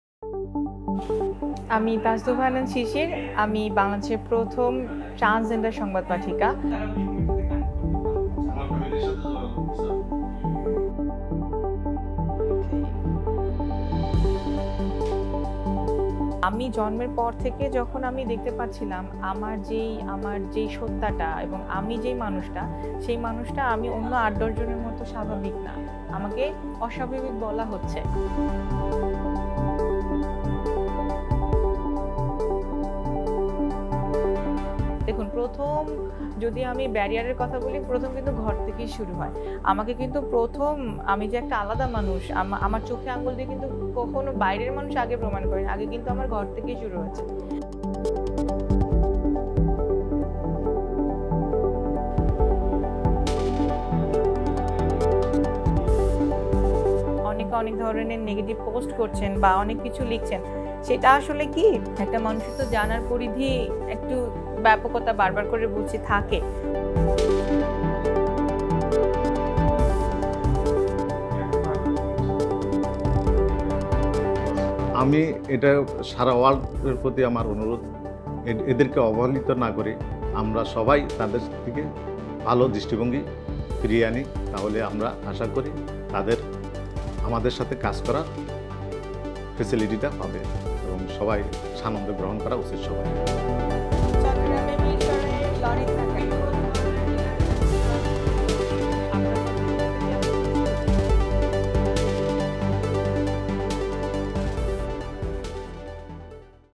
La primera presentadora trans de Bangladés
“La primera barrera a la que me enfrenté en mi vida fue mi propio hogar”. Tashnuva Anan Shishir no recibió ningún apoyo durante su transición, pero eso no le impidió cumplir su sueño de ponerse delante de la cámara.